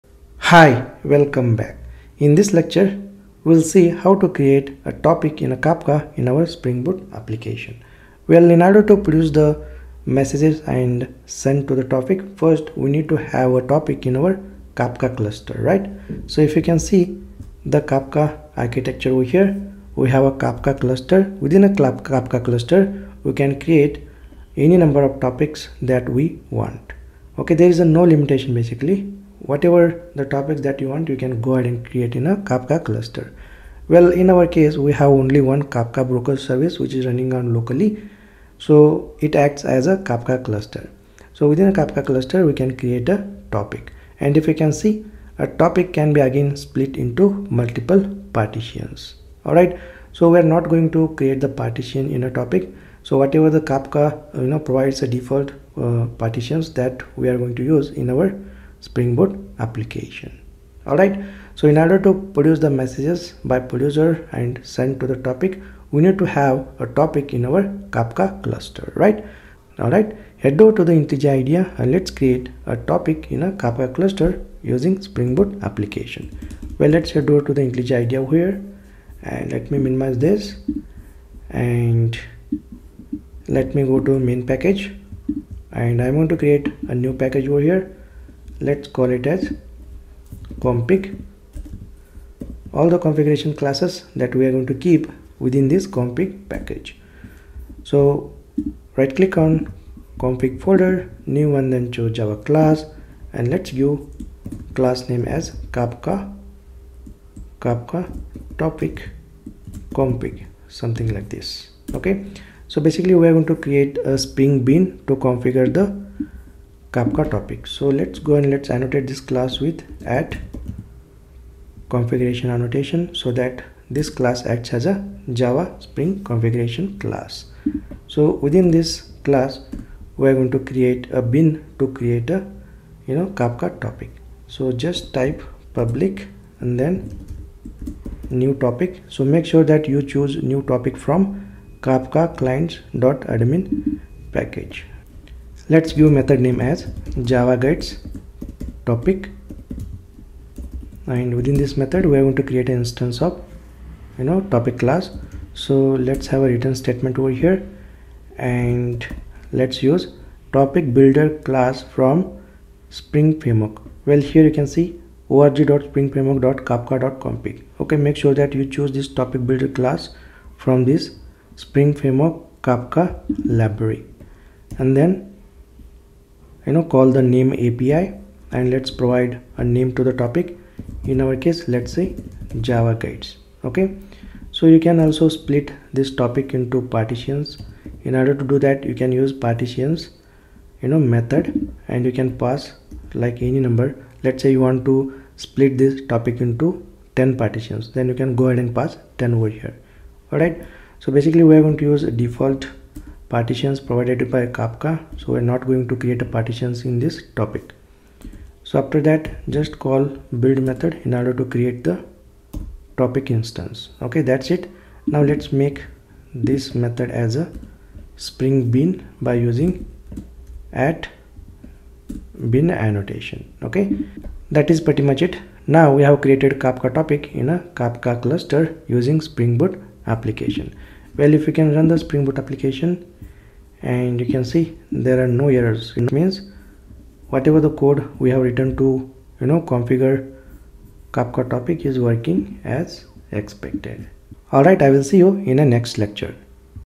Spring Boot + Apache Kafka Tutorial - #6 - Create Kafka Topic
Welcome to Spring Boot + Apache Kafka Tutorial series.
In this lecture, we will create a Kafka topic in our Spring boot application.